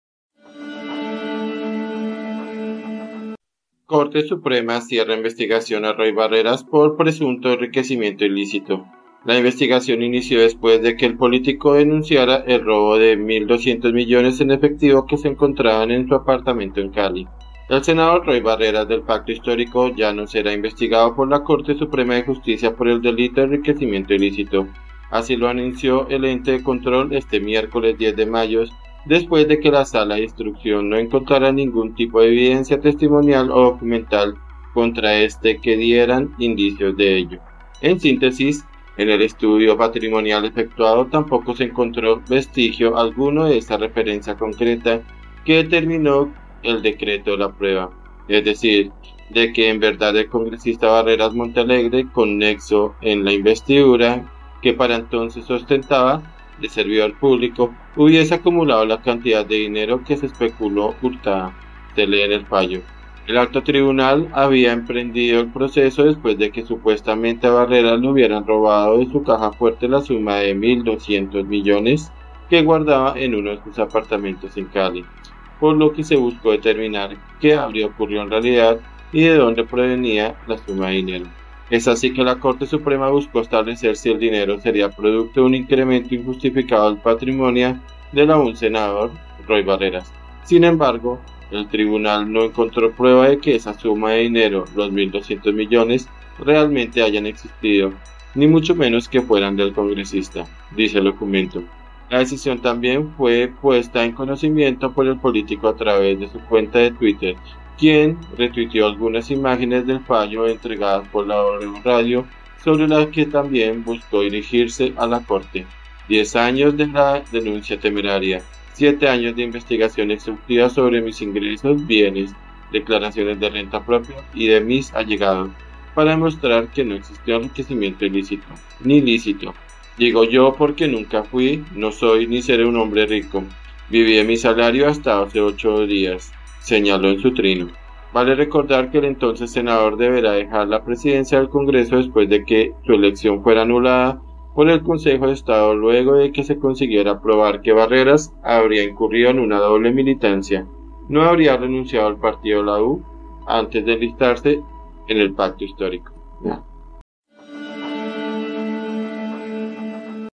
LOÚLTIMO 🔴 CORTE SUPREMA CIERRA INVESTIGACIÓN CONTRA ROY BARRERAS
Corte Suprema cierra investigación a Roy Barreras por presunto enriquecimiento ilícito
La investigación inició después de que el político denunciara el robo de $1.200 millones en efectivo que se encontraban en su apartamento en Cali.